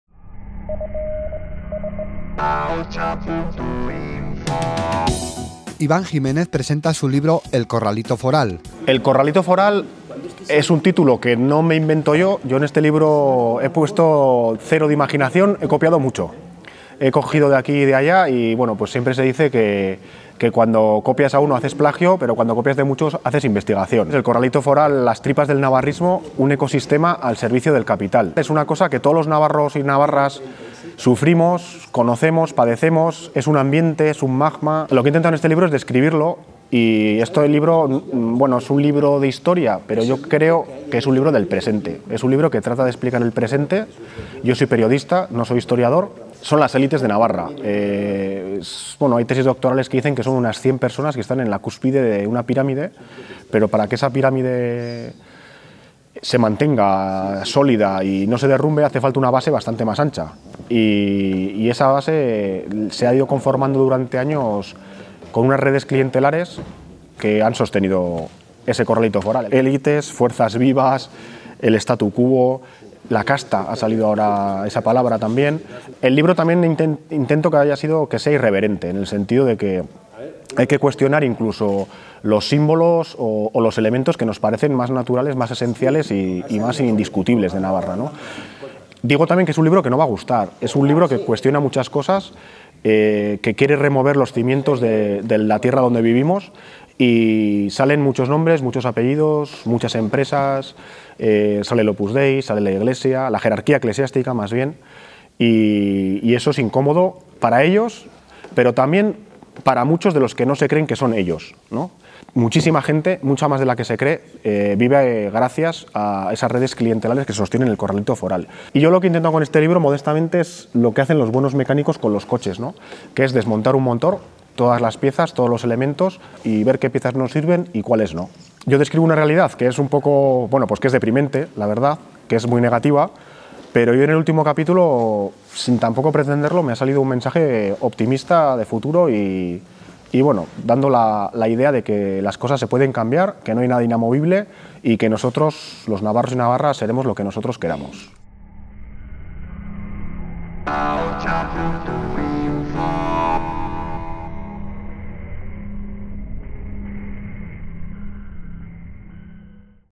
Iván Giménez presenta "El corralito foral", un libro "incómodo" para el régimen
Iván Giménez escarba en este trabajo en "las tripas del navarrismo", y se basa en la historia reciente para explicar muchas de las cosas que suceden en la actualidad. El autor dice que es un libro irreverente, que cuestiona muchas cosas, y que va a ser incómodo para el régimen, porque salen muchos nombres, muchas empresas, el Opus Dei, la jerarquía eclesiástica...